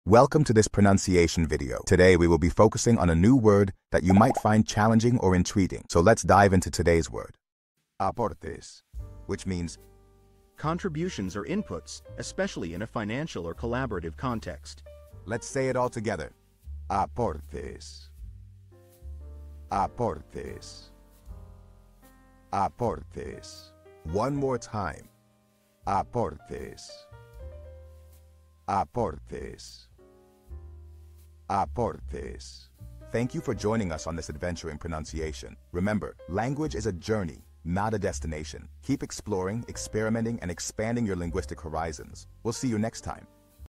How to pronounce Aportes
Master the Pronunciation of 'Aportes which means contributions' - which means : Contributions or Inputs, Specially in a Financial Or Collaborative Context. 📝 with @PronunciationSpanish 🎓🗣️ - [Your Guide to Spanish]

Learn to pronounce 'Aportes' perfectly in Spanish with our easy and engaging tutorial by @PronunciationSpanish! 📖📽️ In this comprehensive video, we provide you with native speaker examples 🗣️👂 and phonetic breakdowns 🎵 to ensure you get the pronunciation just right.

How to say Contributions in Spanish